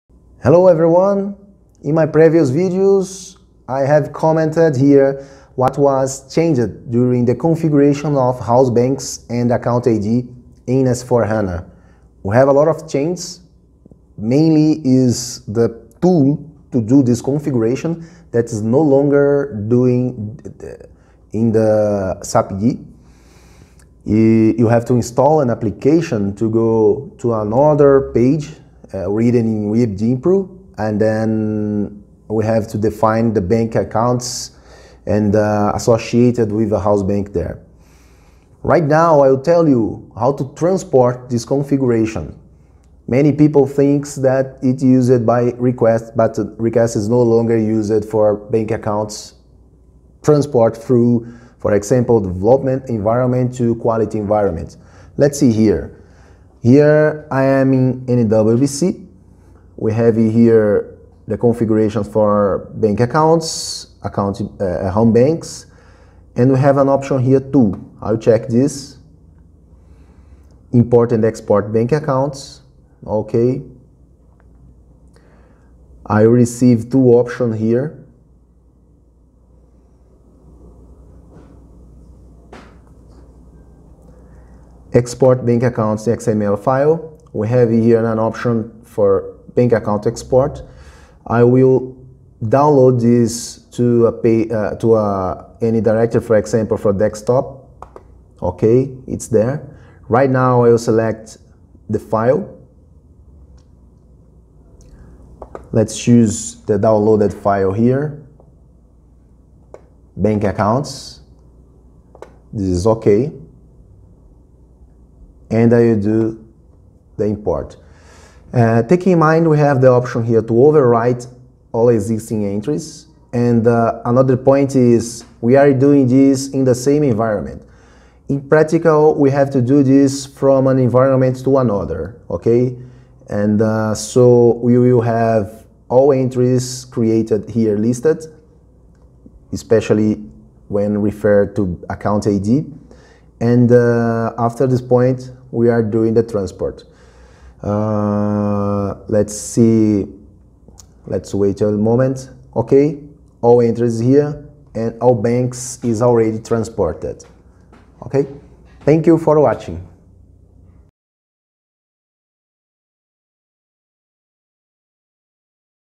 New way to transport Bank Accounts in S/4HANA
In the previous videos we knew the settings was changed for Account ID and House Banks in S/4HANA Finance. In addition, there are changes for data transport of these keys. It is no longer possible to generate Request for transport between environments so we have to exchange XML files.